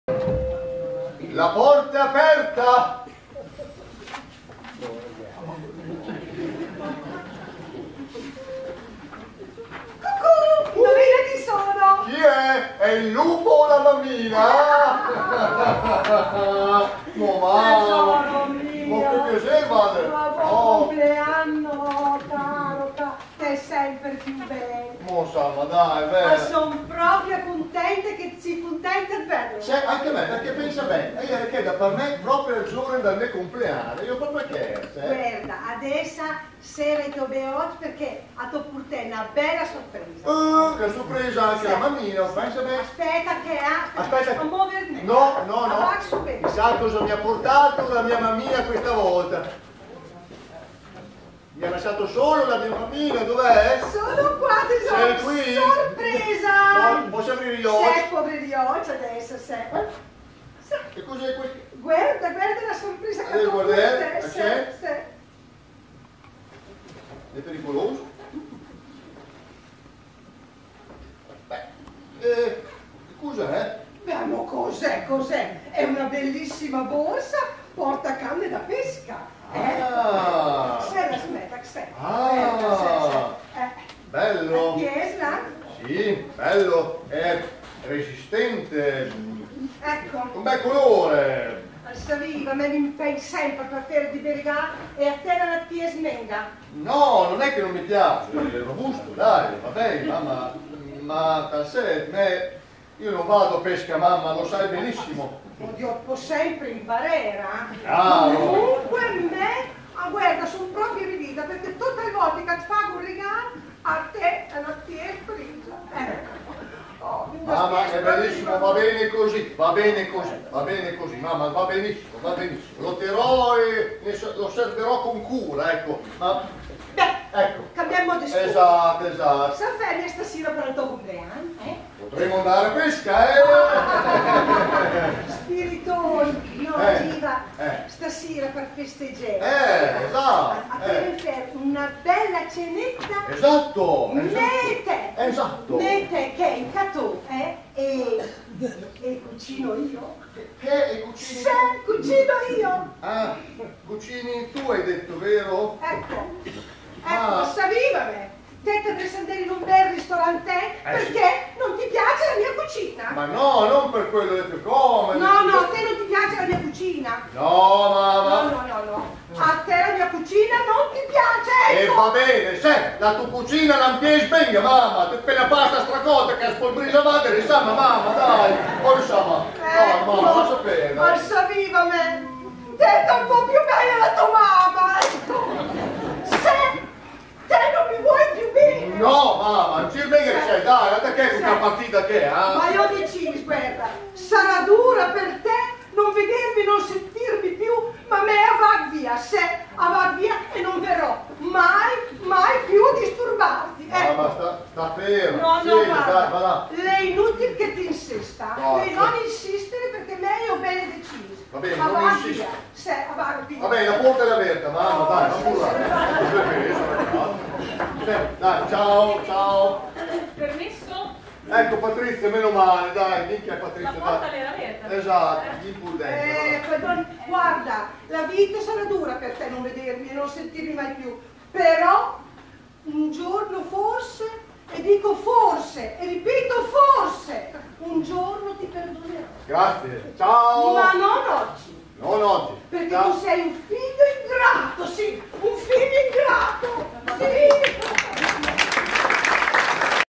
MVI 8373 GLI ARTRISTI - Sàta a chi tàca " Alberto e la mamma
commedia in dialetto modenese
 "Sàta a chi tàca " ( sotto a chi tocca )
interpreti della scena : Elide Marchetti la mamma e Leonardo Greco il figlio.